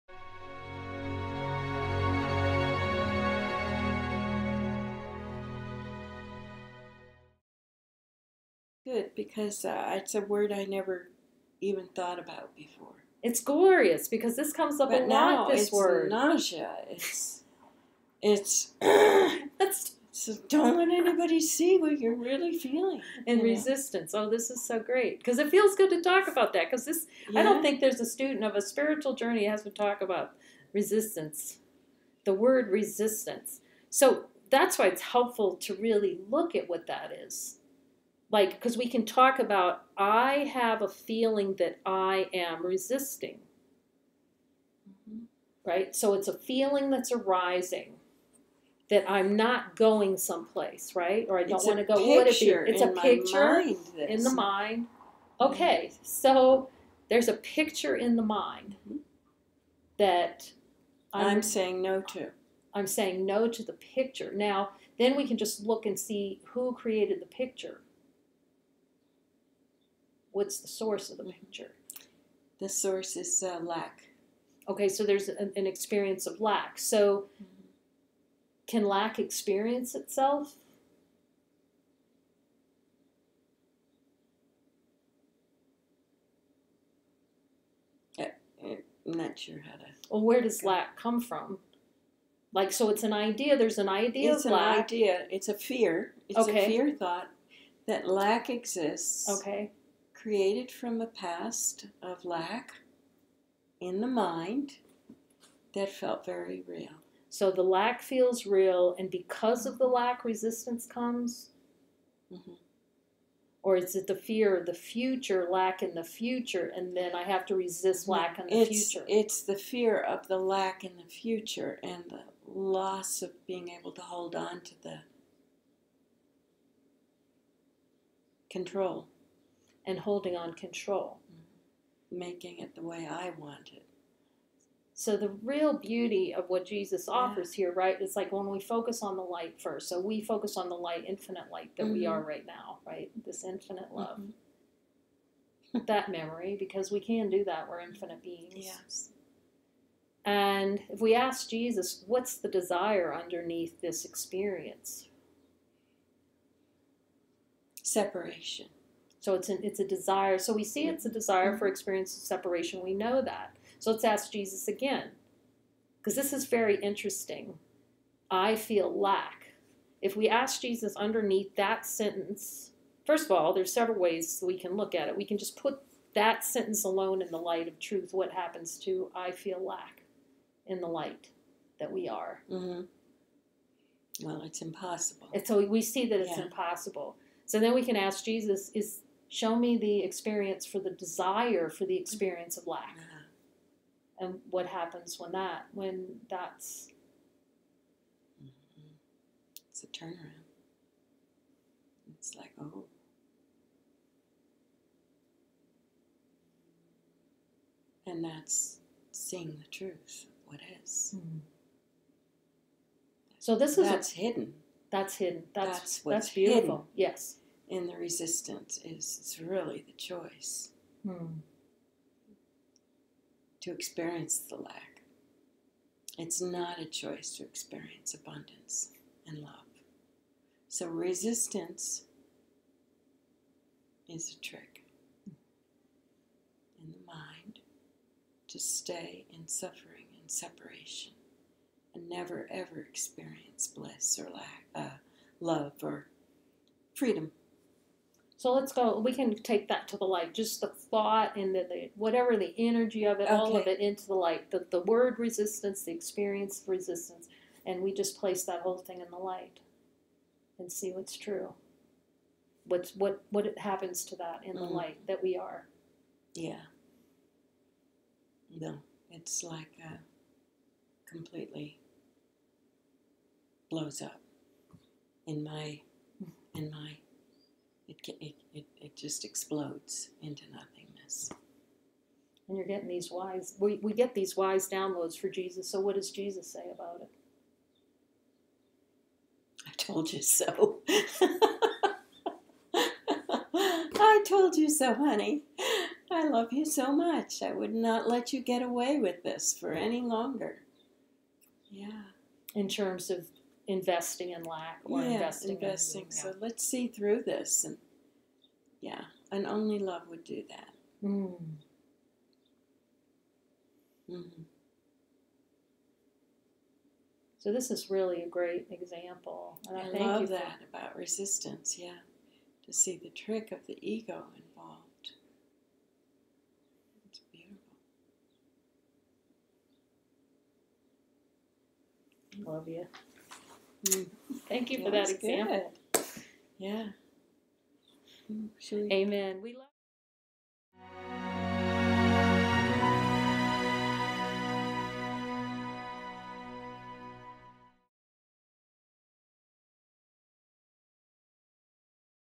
Mini Light Circle - The trick of Resistance To Spiritual Awakening.
In this one-on-one mini light circle, we look at the idea of resistance that seems to arise in spiritual awakening. What does resistance mean?

This is a new look at resistance in the light of truth- Divine Being- and how resistance is the Ego's way of denying divinity is here now. This is an example of how quick the correction (in ACIM this is called forgiveness and in other non-dual teachings use the world Satsang) is with the willingness to see what is true.

This video explores how quickly the power of the Light can see through resistance.

In Jesus: A New Covenant, Jesus points to the power of direct experience of the Light within to clear blocks. In Light Circles beliefs and desires that seem to block Awareness of unconditional Love, our true nature,  are quickly cleared.  And if asked, Jesus will reveal the subconscious blocks.  This principle is being applied daily at 11 am via Zoom to ACIM Course lessons and at 7 pm for general beliefs and desires.

This demonstrates the speed and power of looking directly at beliefs and desires with Jesus- to focus on light and follow the light within as the speedup for Spiritual Awakening.

Drop all metaphysical discussions and explanations. You are Light and as Light you can see the truth of who you are now, and through the power of who you are beliefs can be dropped immediately. Jesus will quickly reveal every belief in the subconscious mind - the chaff that needs to be burned to reveal the good wheat- Unconditional Love.

Can beliefs be cleared quickly?
Yes!